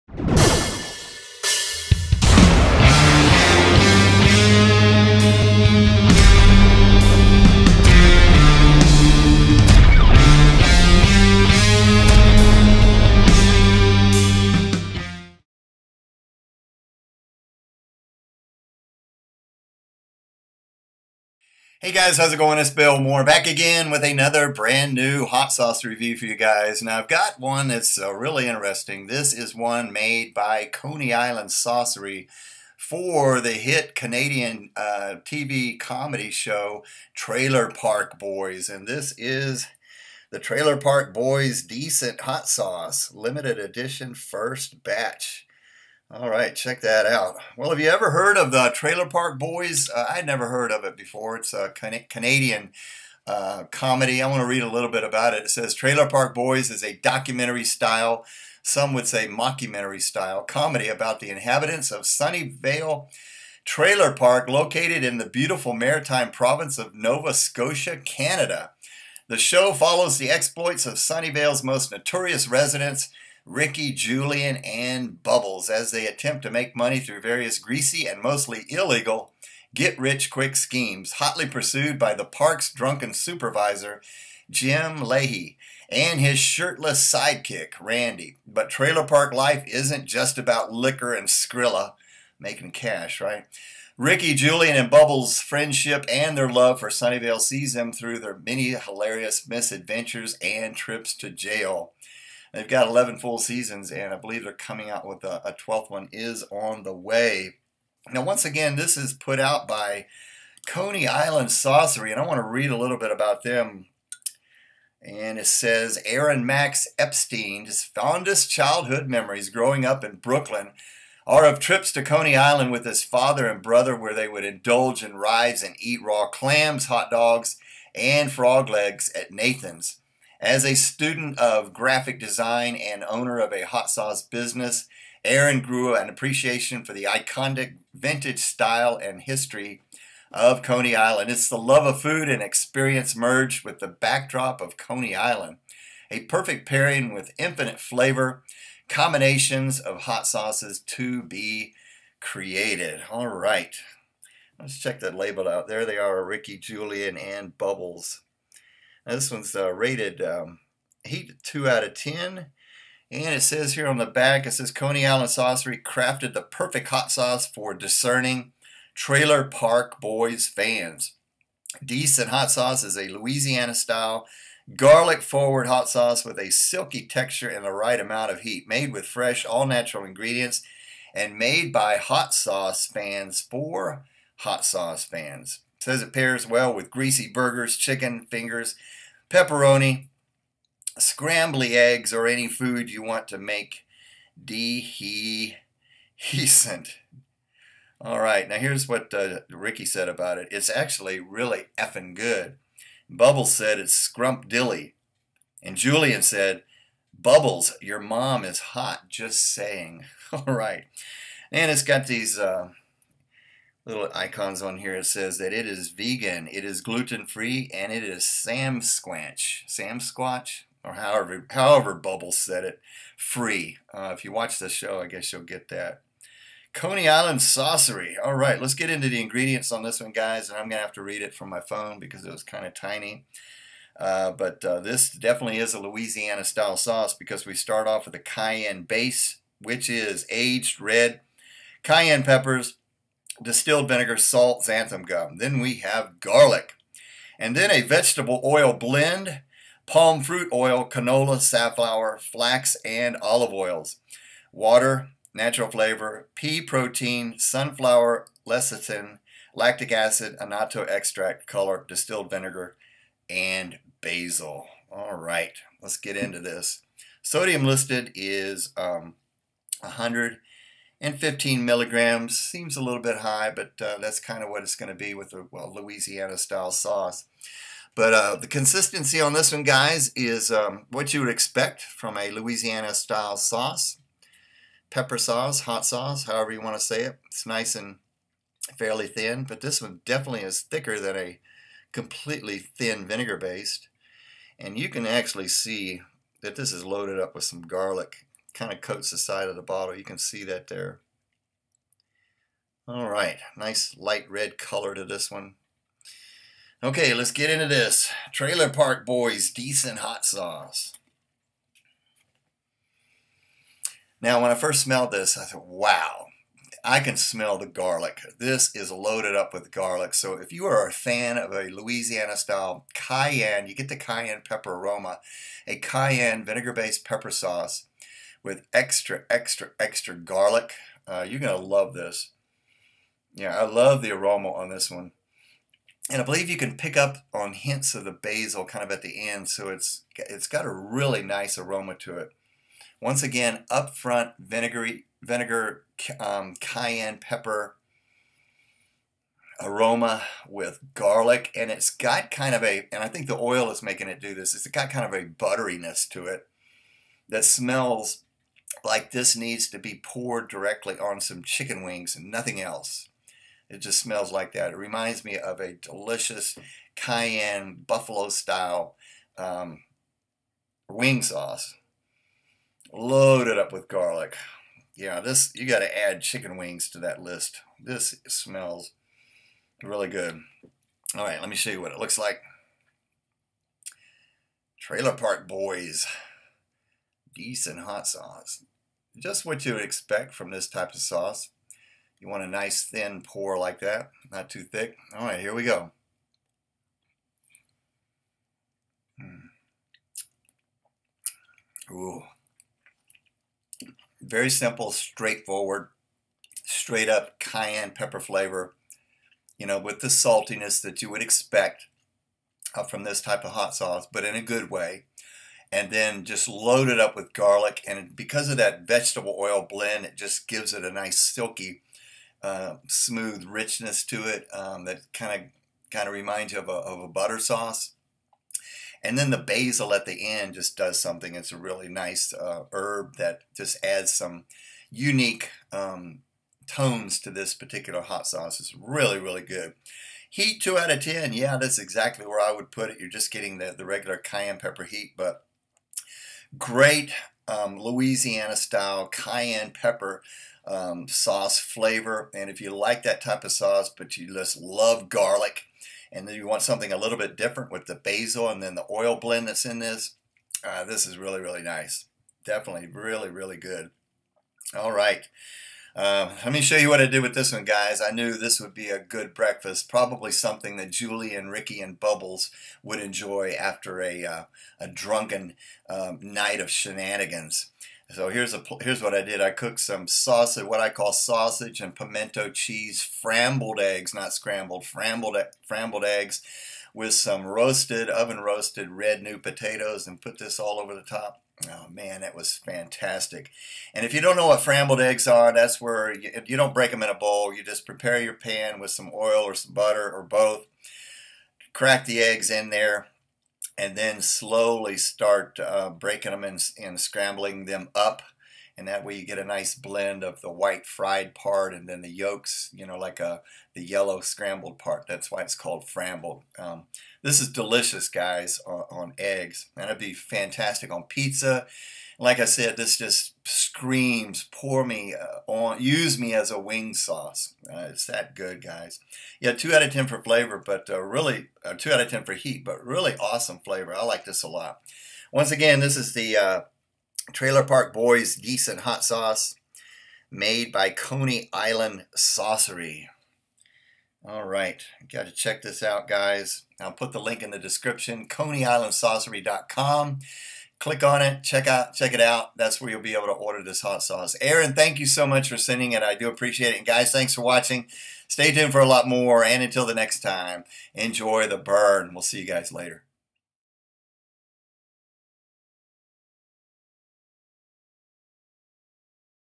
Coney Island Saucery Trailer Park Boys "Deeecent HOT SAUCE" Limited Edition First Batch Review
This can really help me to improve my channel!